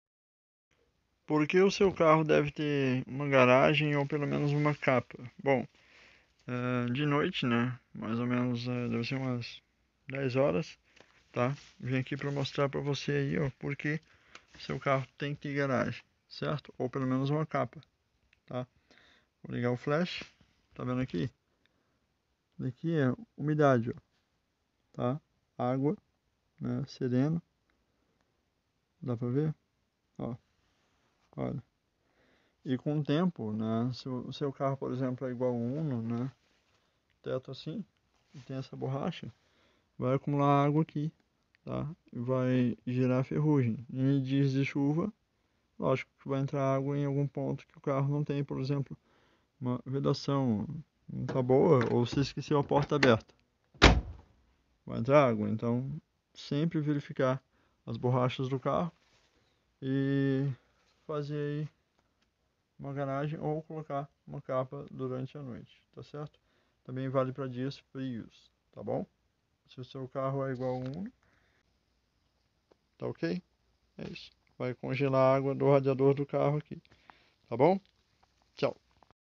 Porque o carro tem que ficar na garagem?
Carro sem garagem olha só o que acontece

Porque carros enferrujam tanto? e como um carro pode entrar água dentro e como evitar entrada de água no carro

Pontos de entrada de água no carro
Grade de ventilação do carro entrando água dentro do carro como resolver, verifique as vedações do ventilador do carro, geralmente entra água no ventilador do carro

Entrada de água na borracha do vidro do carro, quando o carro fica sem garagem, ou o carro fica muito na rua, as borrachas vão dar defeito e com isso vai entrar água no carro

Veja no vídeo outros defeitos de um carro sem garagem